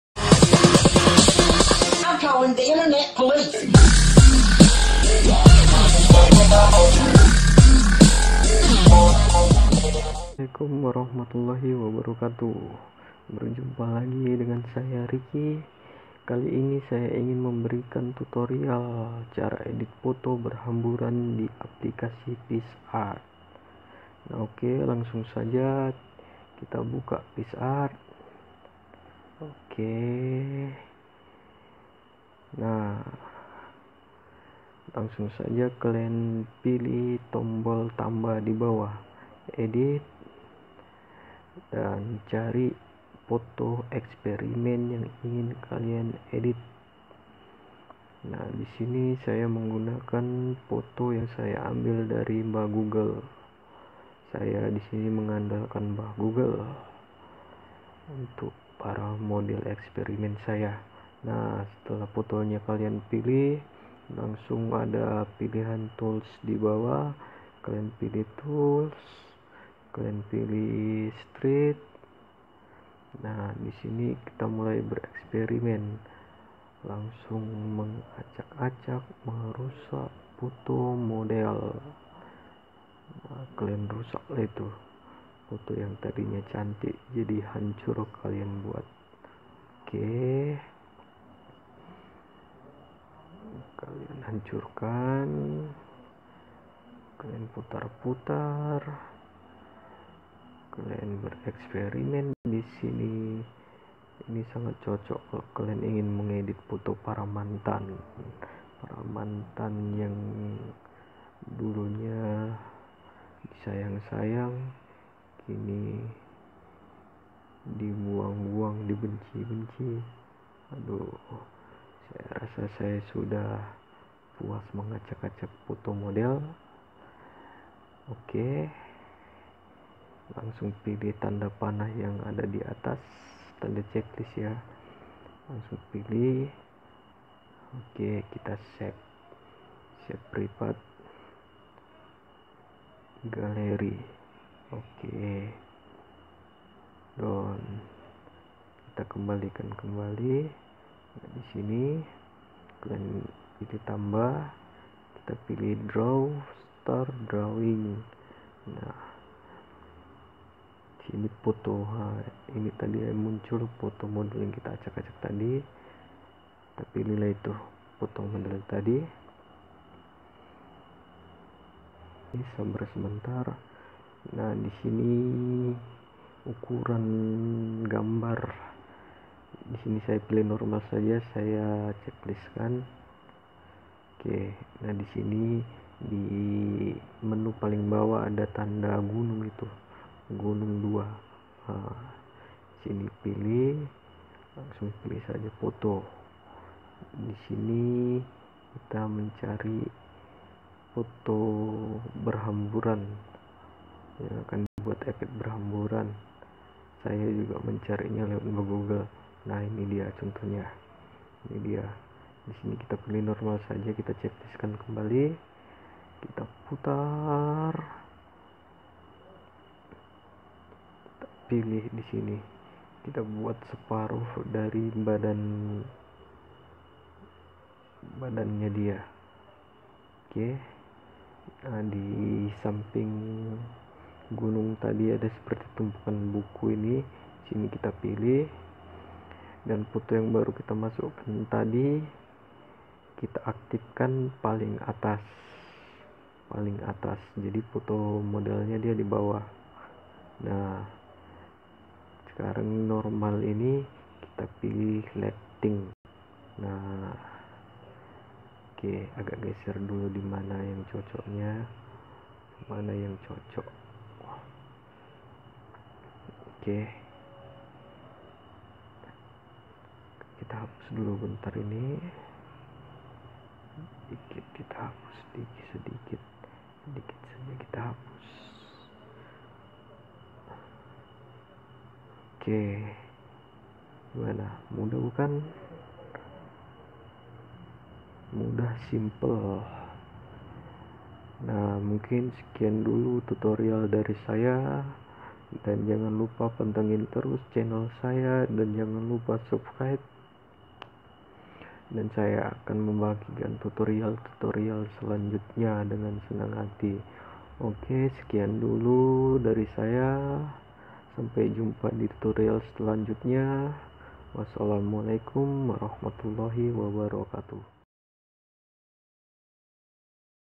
EDIT FOTO BERHAMBURAN DENGAN PICSART | BEST EDITING
Belajar edit foto berhamburan dengan picsart
semua bahan manipulasi yang saya pakai hasil pencarian dari mbah google ya :)